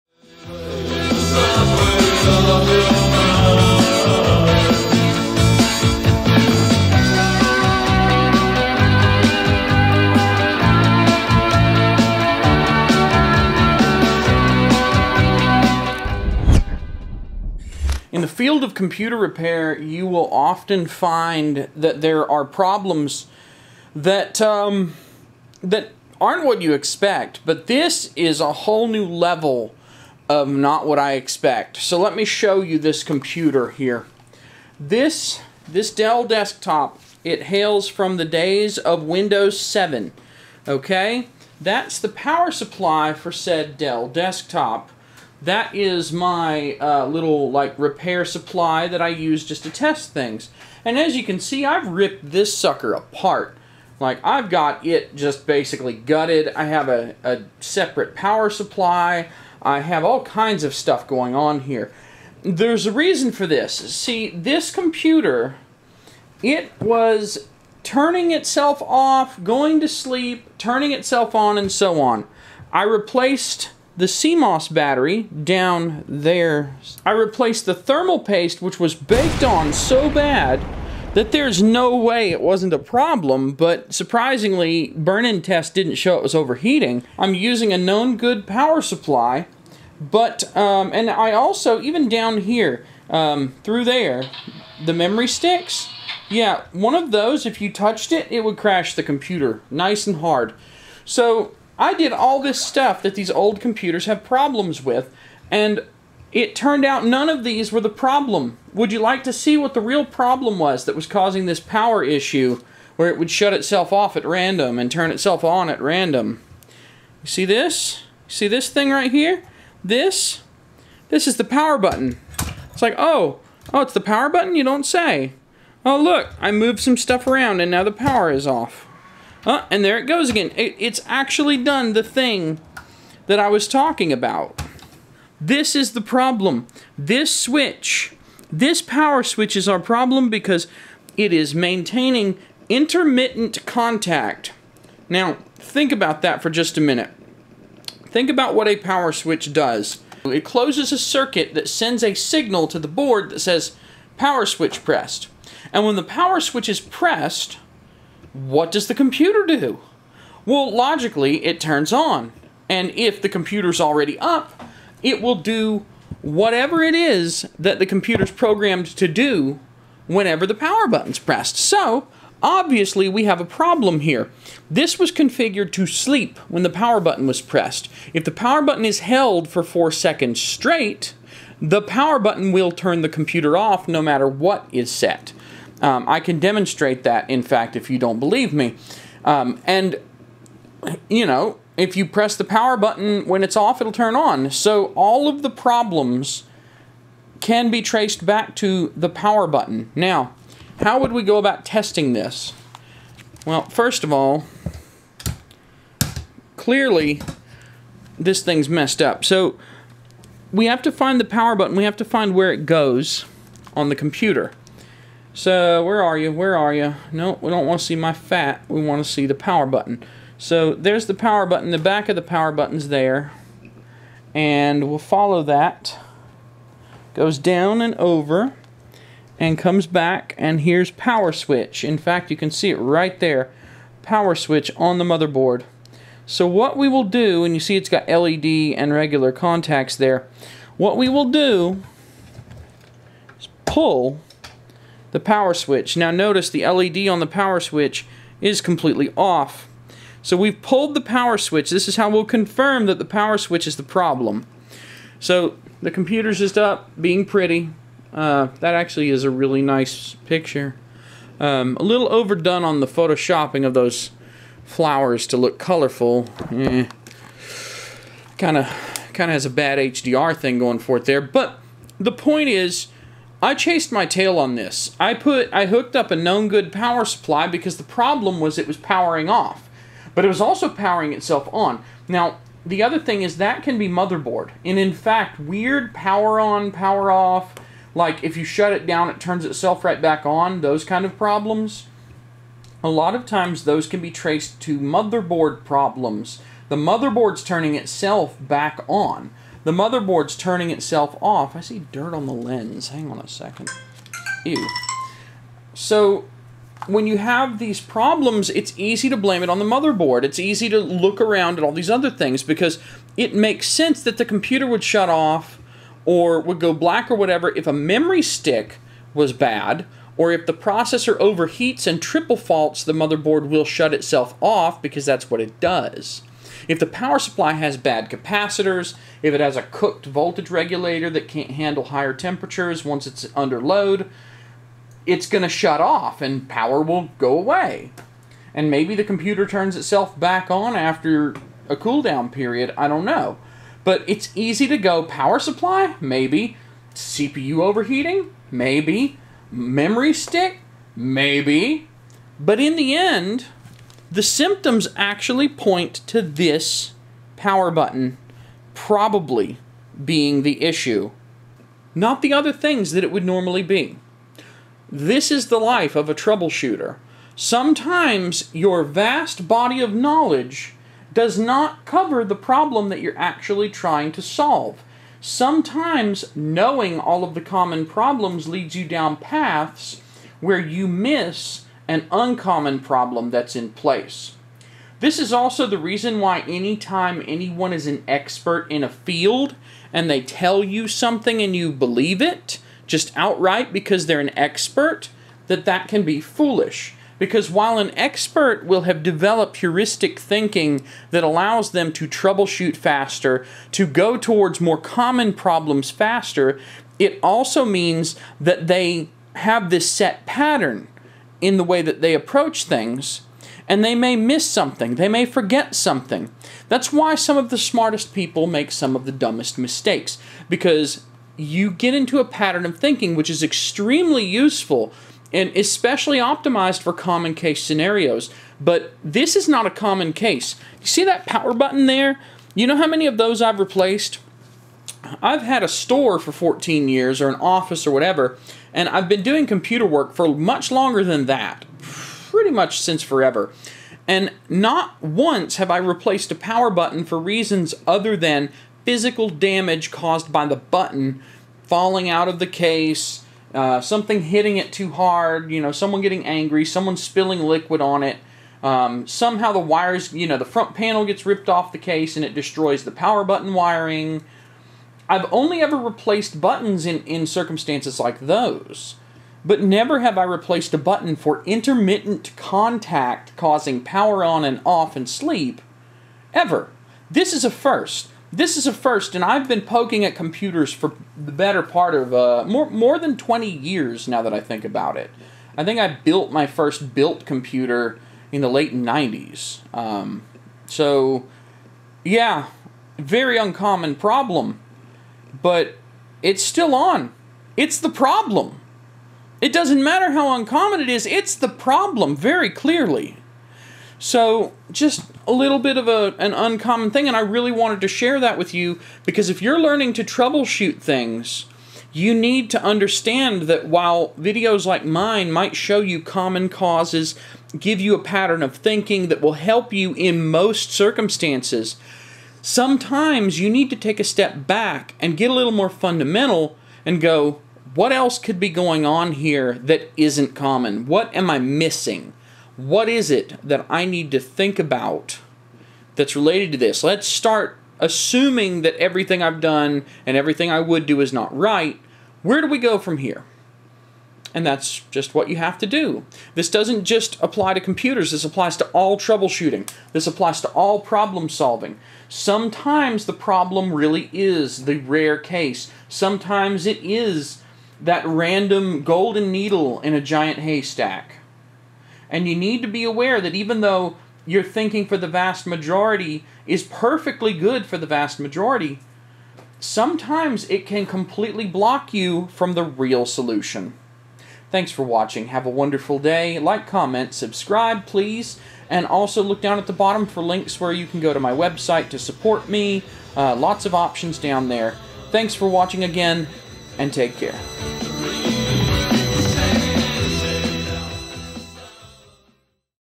The Power Switch Was The Problem The Whole Time - Jody Bruchon Tech
What do you do if a PC shuts itself off? Look at the stuff that might cause a crash, of course. This time...it was none of those things. The power button was making intermittent unwanted contact.